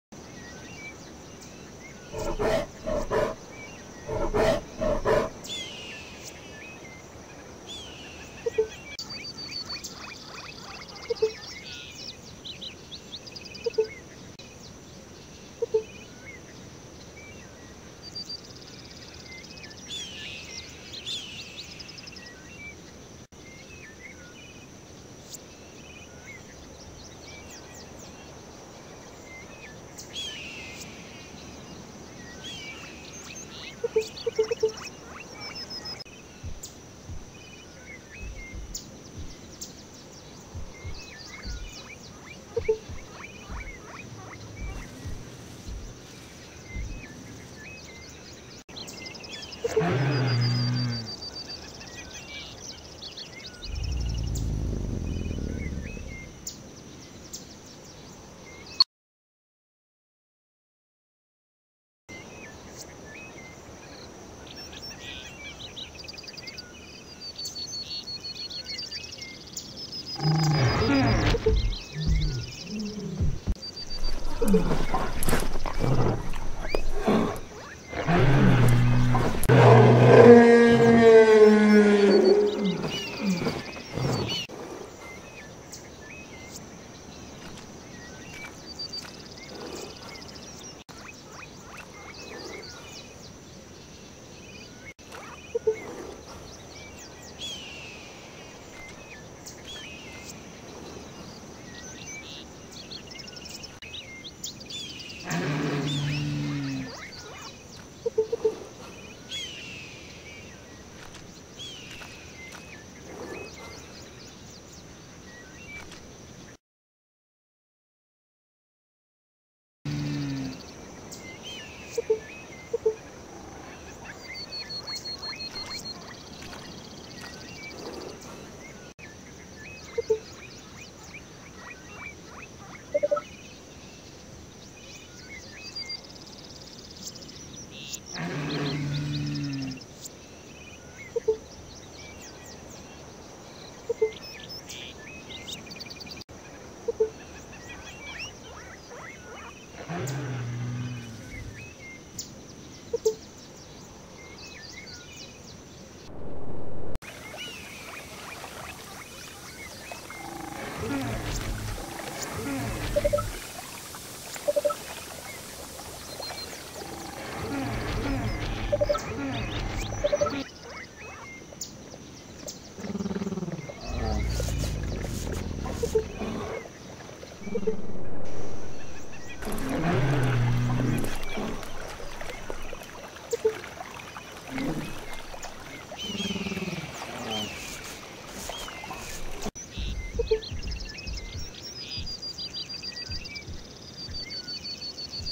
Prehistoric Portals - Parasite Playground Part 2 (Link for Part 1 in Description)
The matriarch of the Triceratops herd desperately tries to get rid of its parasite as the portal starts to disappear.